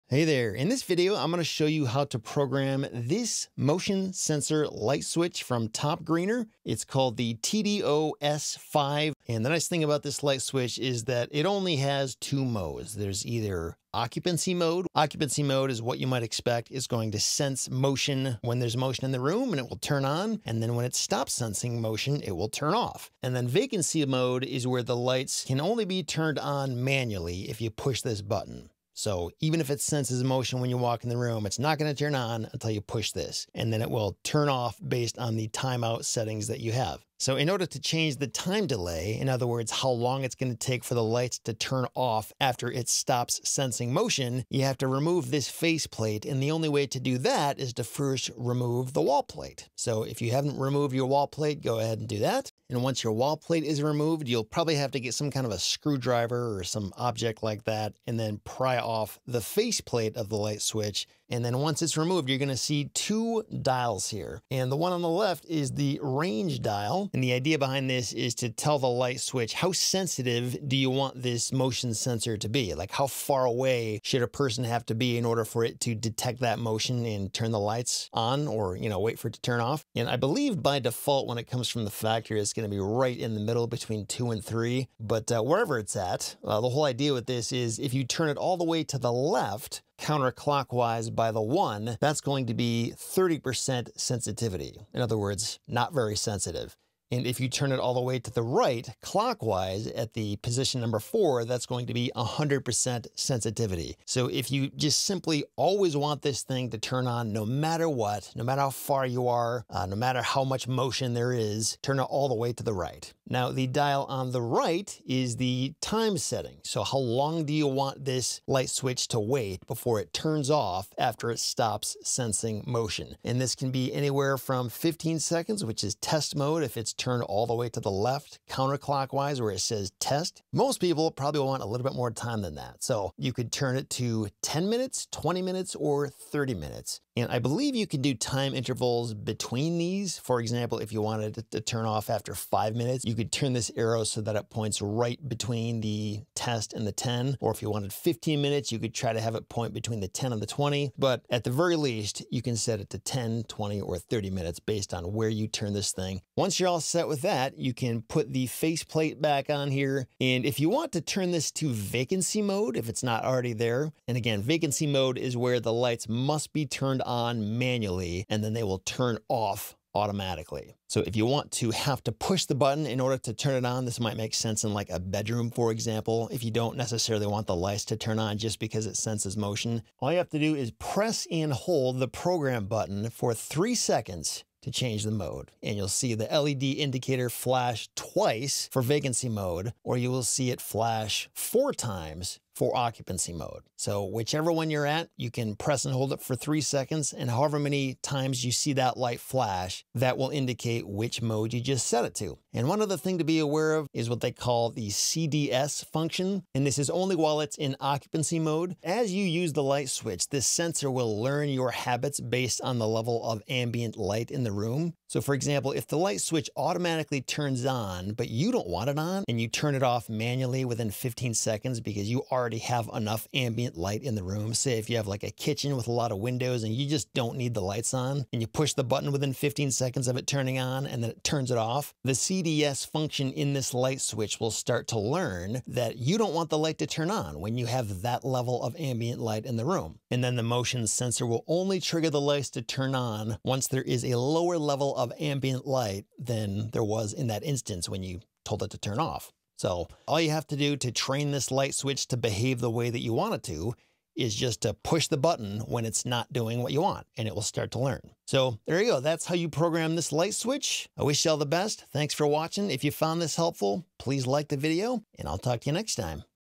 TopGreener TDOS5-KM-W Motion Sensor Switch Programming & Setup Instructions
0:00 - Intro
0:14 - Occupancy vs Vacancy Mode
0:45 - How to Access the Controls
1:04 - Adjusting the Range Settings
1:54 - Adjusting the Time Delay Settings
2:41 - Switching Between Vacancy and Occupancy Mode
3:22 - What Is the CDS Function?

Do you have one of these motion sensor light switches from TopGreener? If so, I'll show you how to adjust the motion range detection and time delay settings in this programming and setup video.